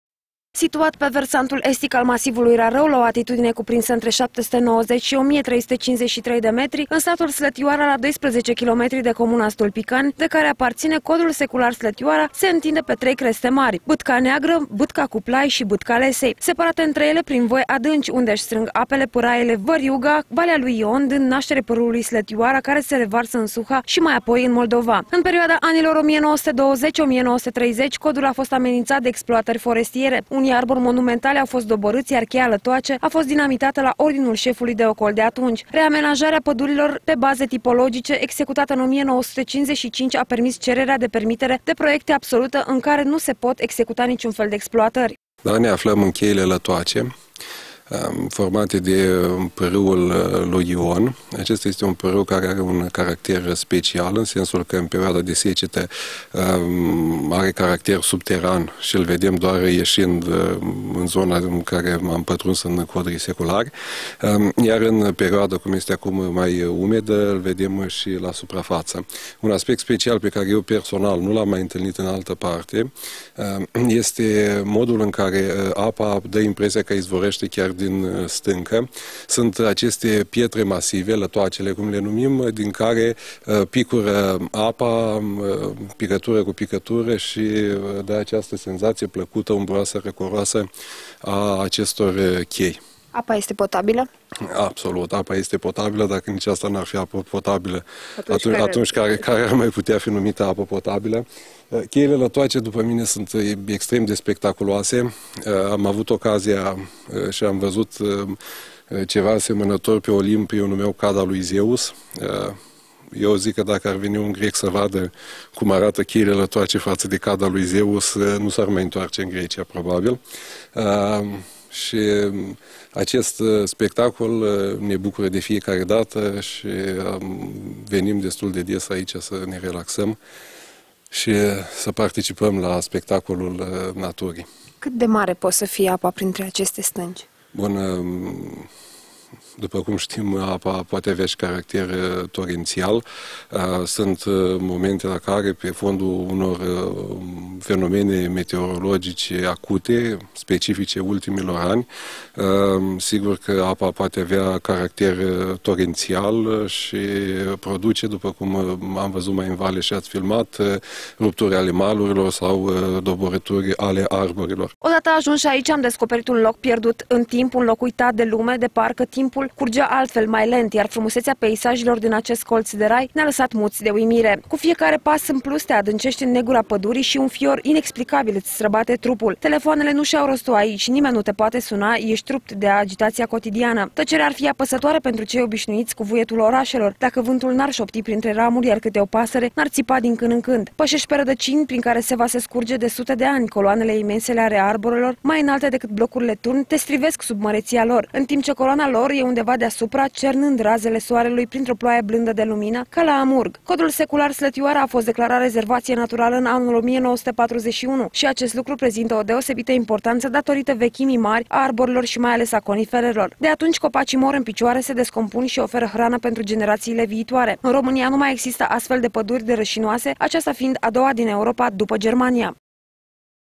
Cheile Latoace - Minune Bucovineană
Dacă nu ați vedea, nu ați crede ca în inima Bucovinei se ascund adevărate colțuri de Rai. Pe tărâmul veşniciei, în Codrii Seculari de la Slătioara, poveşti vechi de sute de ani stau scrise în inelele arborilor. Printre sutele de minuni ale naturii,  Cheile Latoace uimesc prin mareție și splendoare.